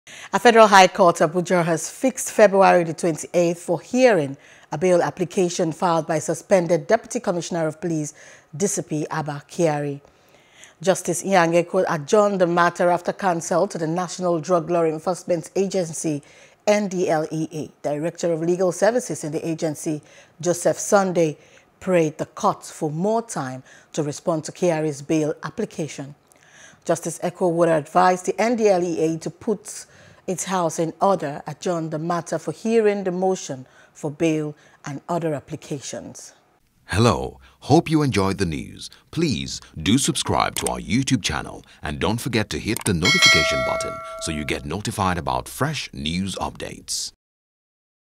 Abba Kyari: Court Fixes Feb. 28 For Bail Application | NEWS
A Federal High Court in Abuja has fixed the 28th of February for hearing a bail application filed by suspended Deputy Commissioner of Police (DCP) Abba Kyari.